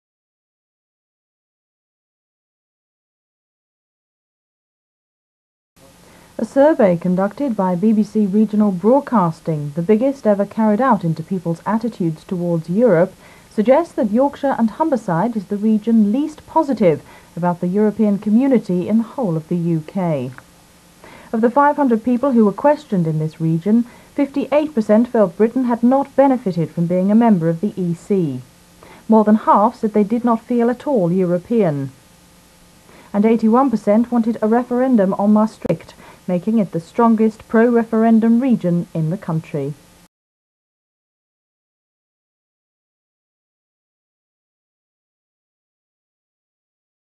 Brexit Referendum Denied in 1992 even though 81% demanded one.
In 1992 PM John Major forced through the watershed Maastricht Treaty which unified Europe and turned it from a trading bloc into a Sovereign State with the intention of building a European Empire.
There were many calls from the UK population  for a referendum on this crucial EU treaty.  In Yorkshire, described in this clip as statistically the most anti-EU area of Britain a Gallup poll found 81% of people were demanding a referendum.  Major , a fully paid up member of the Elite, refused the people that vote, just as he has today trying to cripple our exit from Europe during Brexit.   How many young people even know that these anti-democratic strategies were used to force the UK population into a Federal Europe against their wishes before many of them were born.  Talk of 'protecting democracy' is the worst kind of Stalinist agitprop from autocratic politicians who have all done well for themselves whilst the needs of ordinary citizens of our country have been forgotten.
In 1992 the Gallup Poll showed that a whopping 58% of people at that time thought that Britain had not benefitted from  EU membership and .